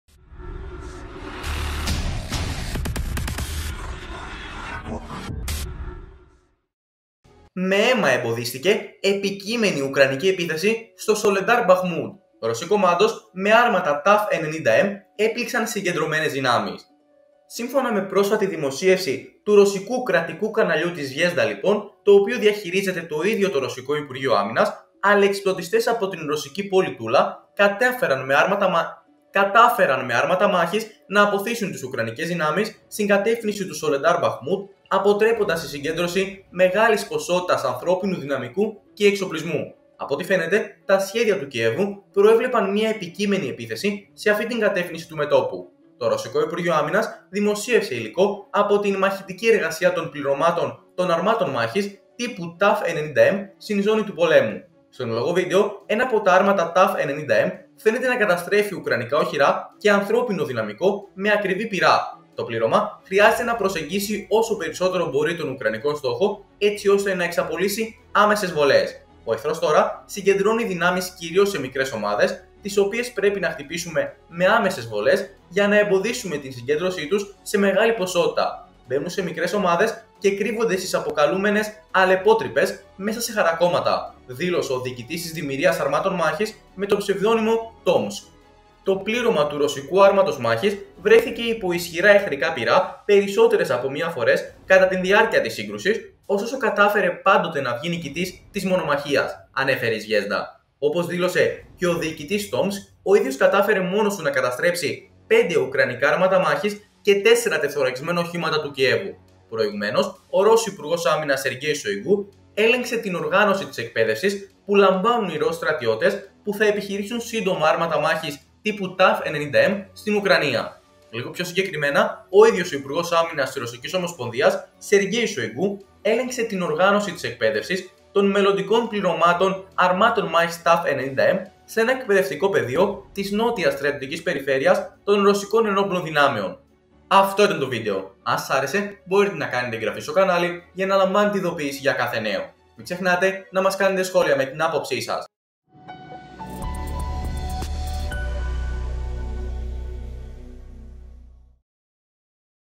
«Καημένοι» Ουκρανοί... Ρώσοι κομάντος έσκασαν με άρματα T-90M σε στρατόπεδο έξω από το Μπαχμούτ...
Με αίμα εμποδίστηκε επικείμενη ουκρανική επίθεση στο Σολεντάρ-Μπαχμούτ!

Ρώσοι κομάντος με άρματα T-90M έπληξαν συγκεντρωμένες δυνάμεις.

Η δράση των ρωσικών τεθωρακισμένων είχε ως αποτέλεσμα την αποτροπή συγκεντρώσεις μεγάλων ουκρανικών δυνάμεων στο σημείο, αποτρέποντας έτσι μία επικείμενη επίθεση σε αυτόν τον τομέα του μετώπου του εν εξελίξει πολέμου.

Βάση αναφορών του Κρεμλίνου, το οποίο διαχειρίζεται το ίδιο το ρωσικό υπουργείο άμυνας, αλεξιπτωτιστές από την ρωσική πόλη Τούλα κατάφεραν με άρματα μάχης να απωθήσουν τις ουκρανικές δυνάμεις στην κατεύθυνση του Σολεντάρ-Μπαχμούτ, αποτρέποντας την συγκέντρωση μεγάλης ποσότητας ανθρώπινου δυναμικού και εξοπλισμού. Από ότι φαίνεται τα σχέδια του Κιέβου προέβλεπαν μία επικείμενη επίθεση σε αυτήν την κατεύθυνση του μετώπου.

Το ρωσικό υπουργείο άμυνας δημοσίευσε υλικό από την μαχητική εργασία των πληρωμάτων των αρμάτων μάχης τύπου T-90M στην ζώνη του πολέμου.

Στο εν λόγω βίντεο, ένα από τα άρματα T-90M φαίνεται να καταστρέφει ουκρανικά οχυρά και ανθρώπινο δυναμικό με ακριβή πυρά. Το πλήρωμα χρειάζεται να προσεγγίσει όσο περισσότερο μπορεί τον ουκρανικό στόχο, έτσι ώστε να εξαπολύσει άμεσες βολές.

«Ο εχθρός τώρα συγκεντρώνει δυνάμεις κυρίως σε μικρές ομάδες, τις οποίες πρέπει να χτυπήσουμε με άμεσες βολές για να εμποδίσουμε την συγκέντρωσή τους σε μεγάλη ποσότητα. Μπαίνουν σε μικρές ομάδες και κρύβονται στις αποκαλούμενες 'αλεπότρυπες' μέσα σε χαρακώματα» δήλωσε ο διοικητής της διμοιρίας αρμάτων μάχης με το ψευδώνυμο Tomsk.
Κεφάλαια:
0:00 - Εισαγωγή
01:52 - Εκπαίδευση Υπό την Επίβλεψη Σοϊγκού